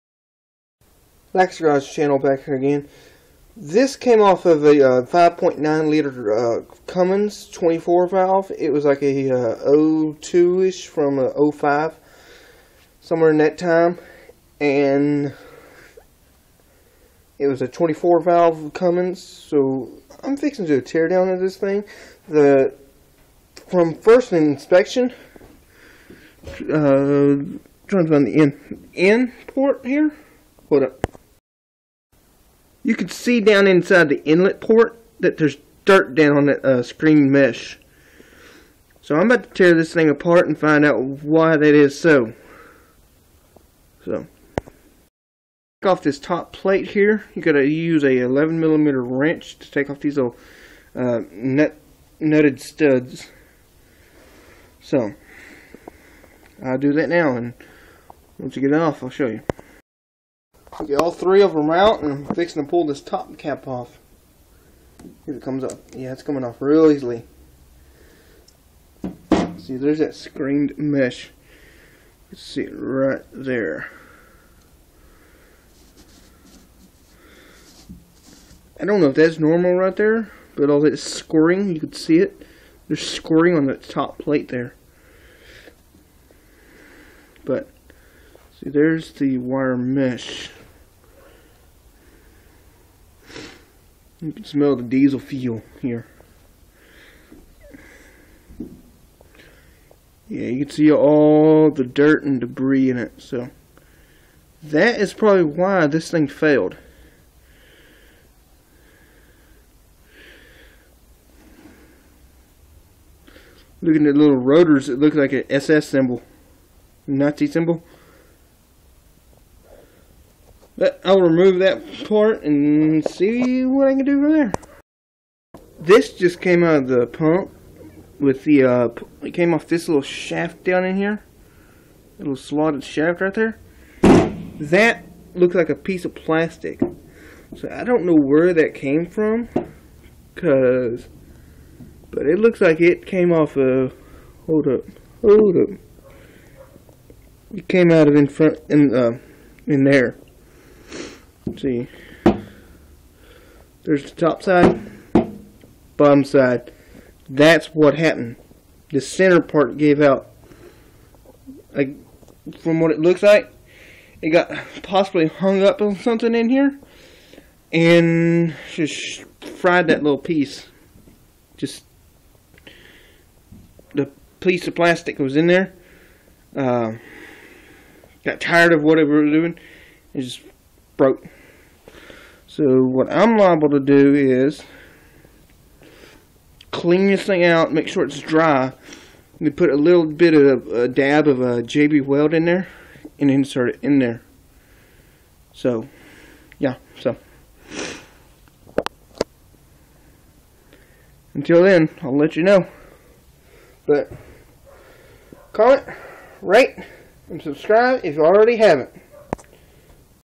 5.9l Cummins fuel lift pump teardown
Tear down of a 02-05 Cummins fuel lift pump, and inspection.